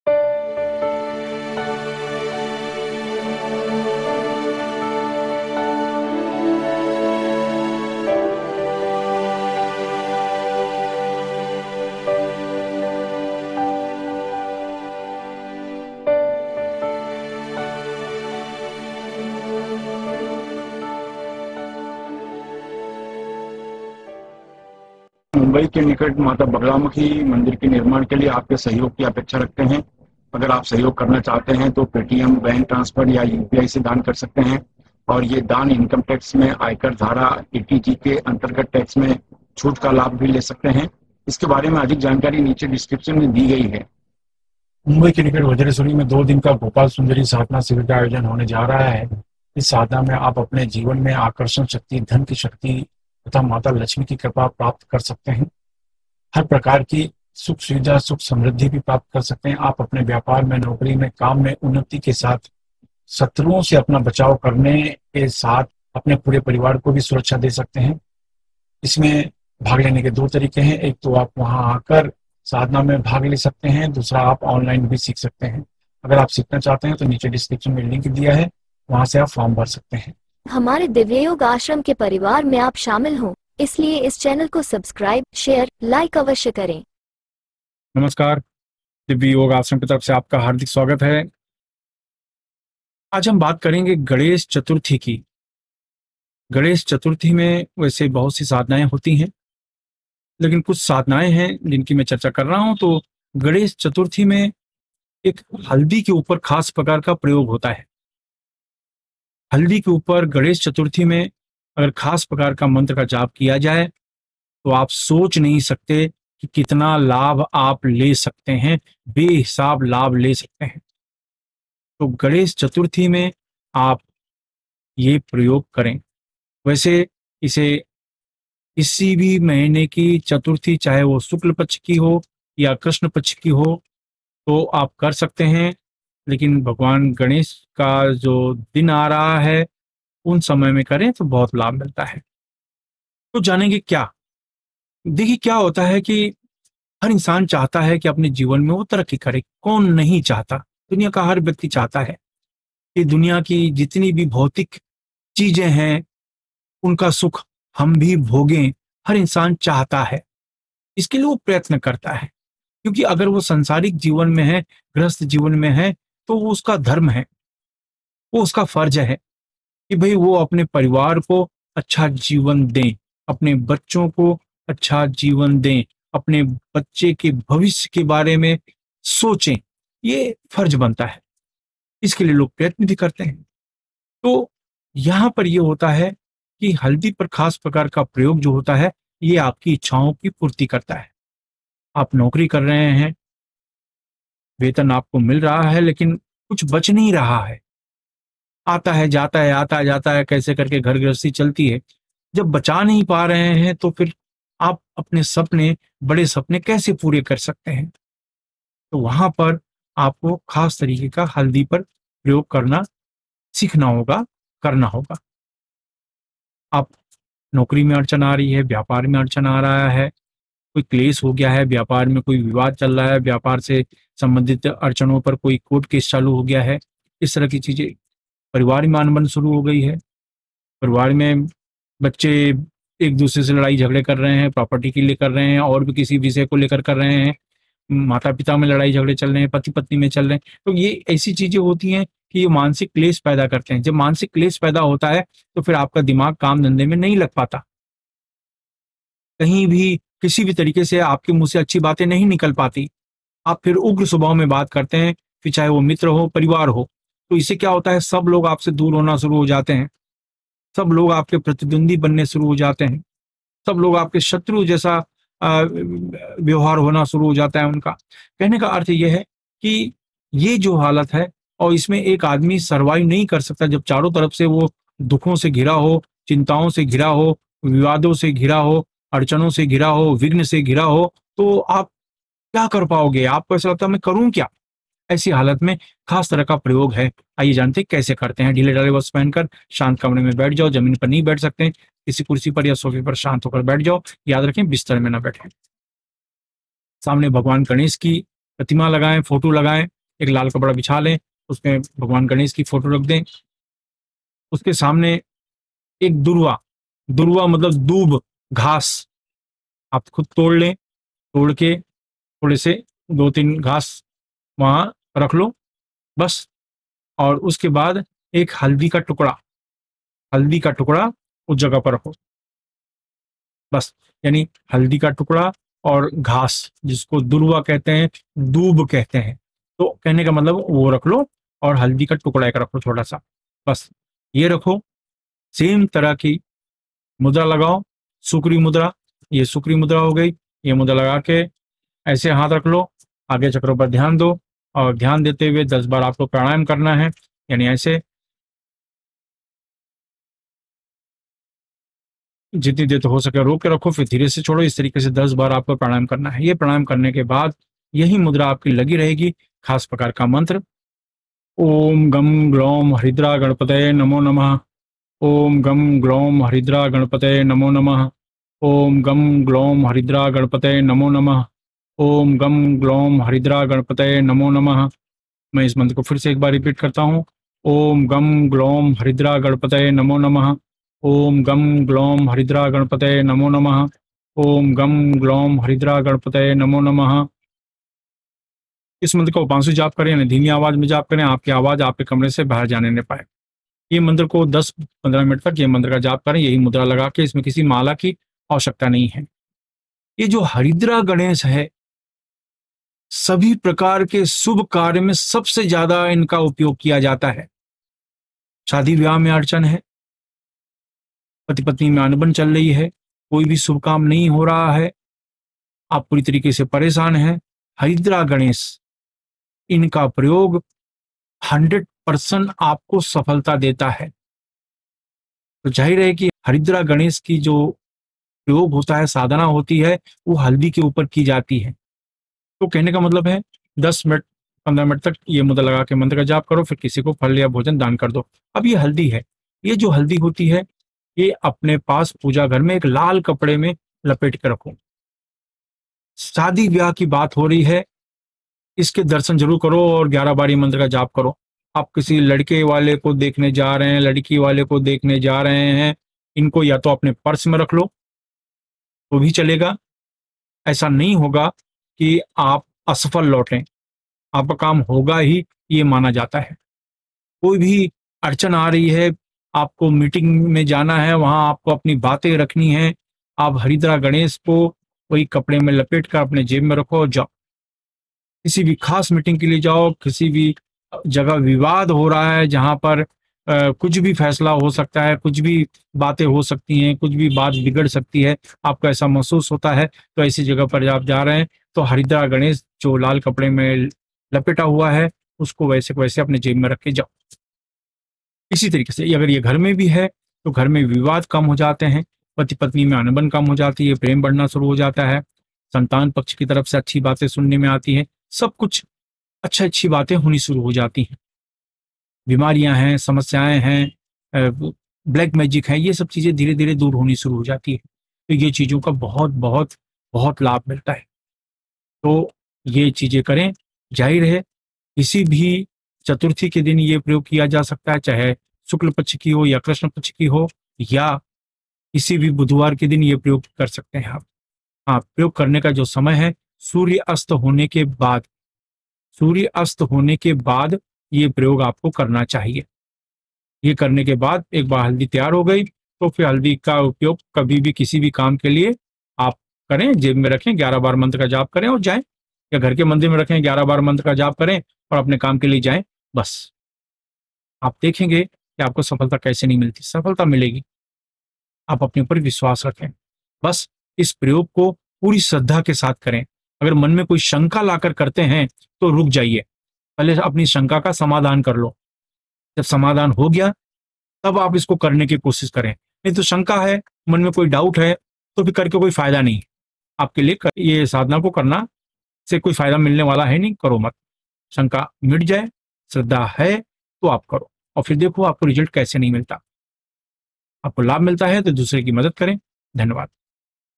किसी भी चतुर्थी को हल्दी का प्रयोग १००% आपकी मनोकामना की पुर्ति करता है अचूक टोटका।
भगवान गणेश का ये प्रयोग हल्दी पर किसी भी चतुर्थी पर करे तो सभी प्रकार की मंगलकार्य मनोकामना पूरी होनी शुरु हो जाती है. Totka Haridra Ganesh mantra- ॥ॐ गं ग्लौं हरिद्रा गणपतये नमो नमः॥ ||OM GAMM GLOUM HARIDRA GANAPATAYE NAMO NAMAHA ||

UPI-            sdashram@icici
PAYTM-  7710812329